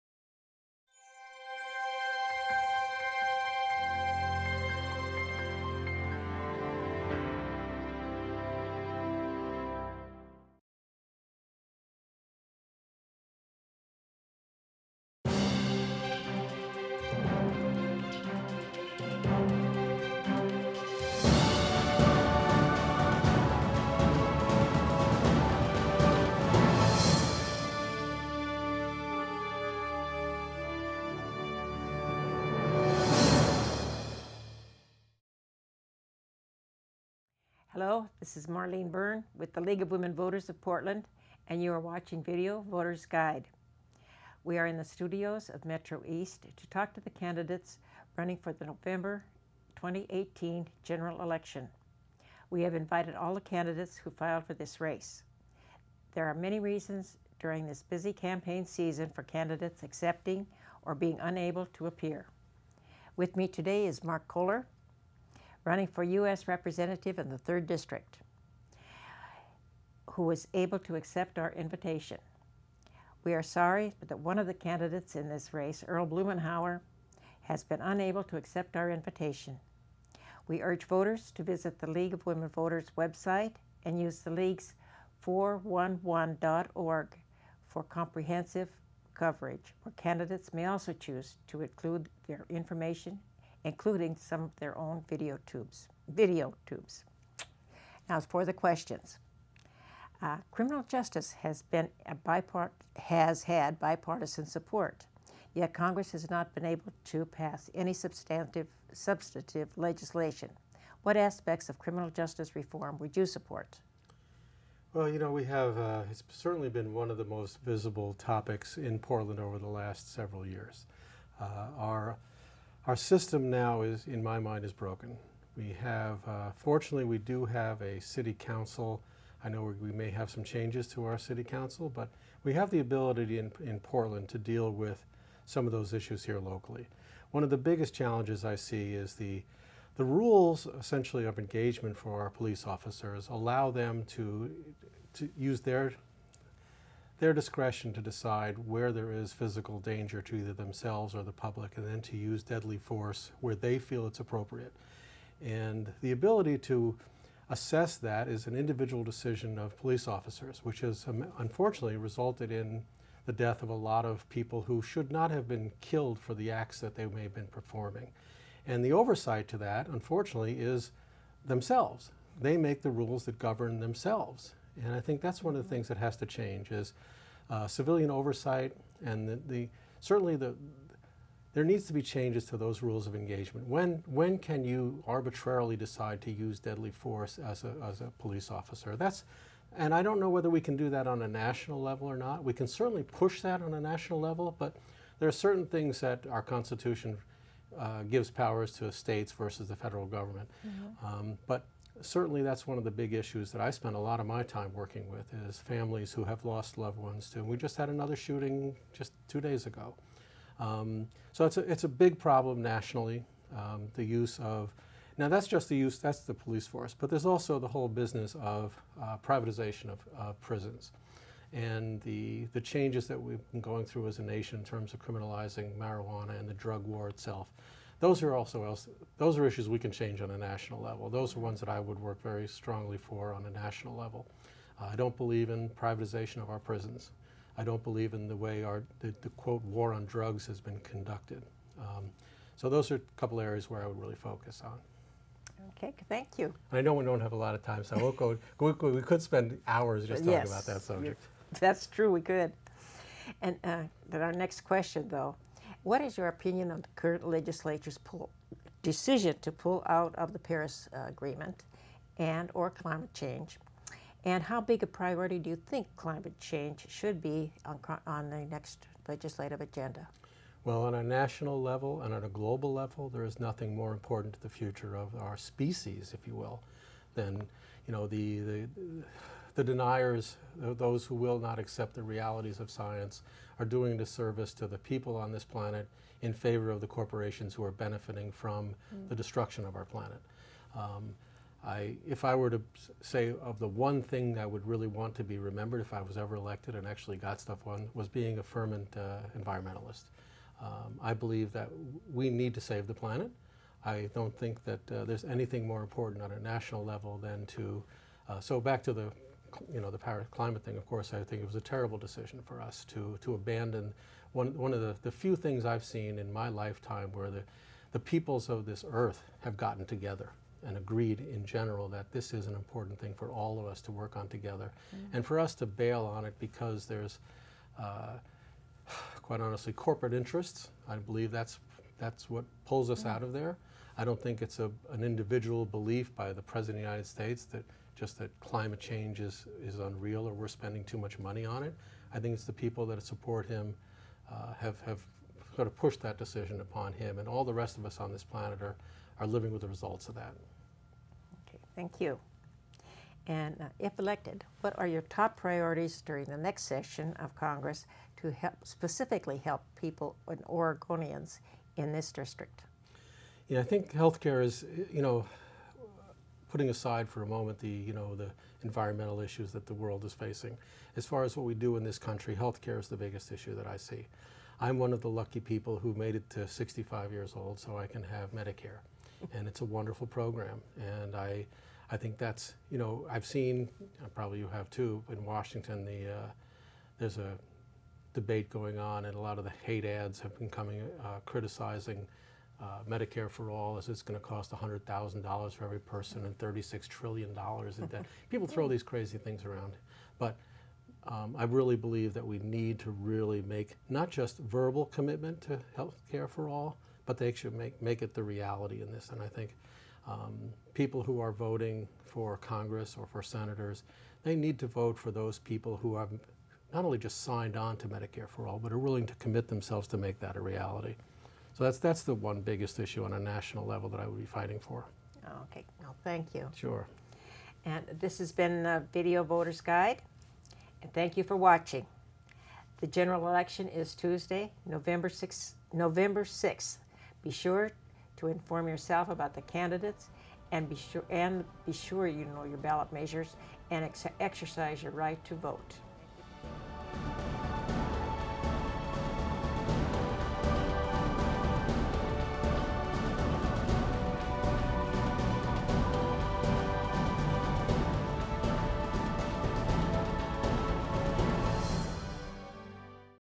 Video Voters' Guide 2018 Marc Koller
The League of Women Voters of Portland and MetroEast introduces you to this election's candidates.